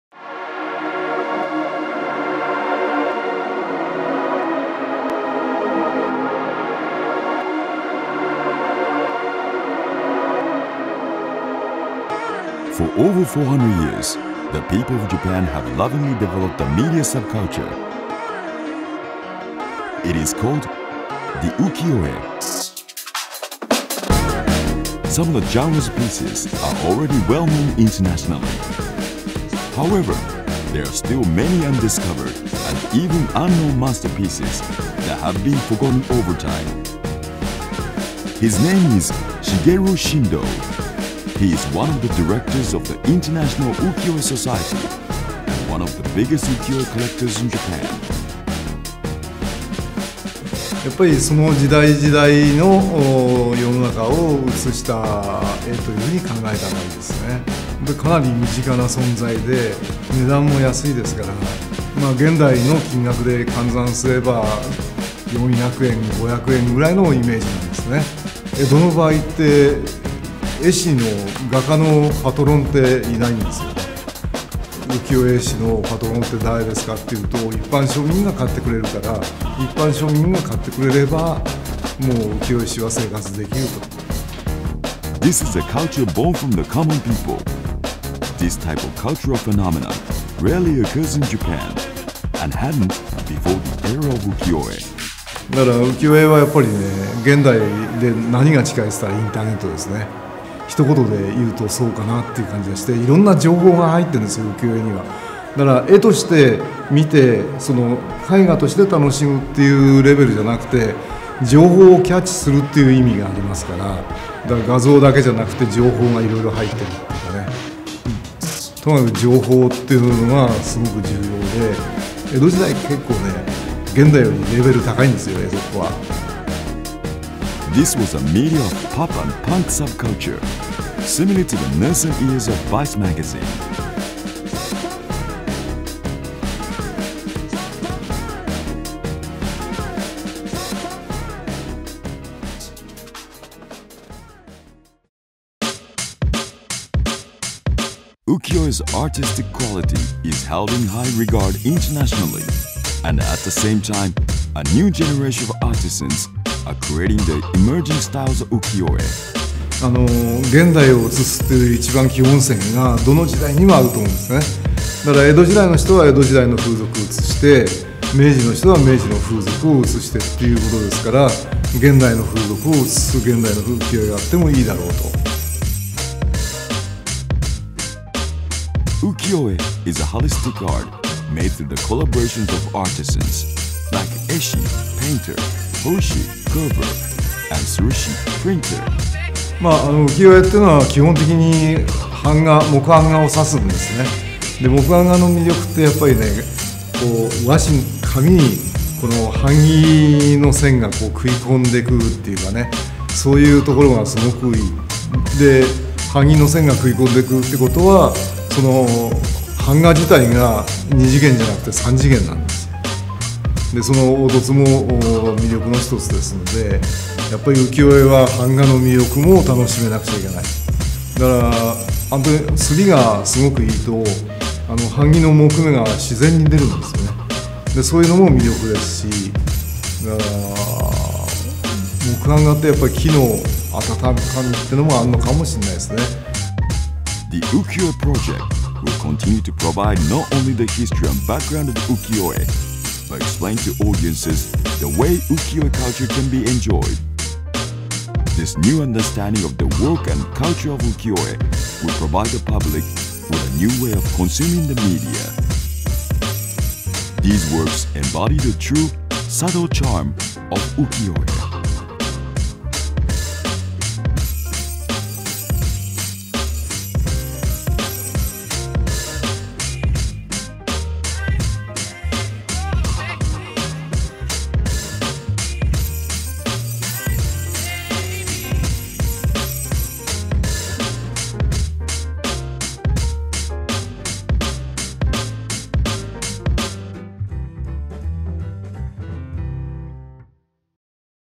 UKIYO-E PROJECT #1 Tokyo × Ukiyo-e People
Discover the true understanding of Ukiyo-E.
‘UKIYO-E PROJECT” will set up  a new hypothesis that would overcome a stereotype of perceiving Ukiyoe culture.
All pieces shown in the movie are all rights received by Mitsui agency,or an original vintage pieces from professor Shindo’s private collection.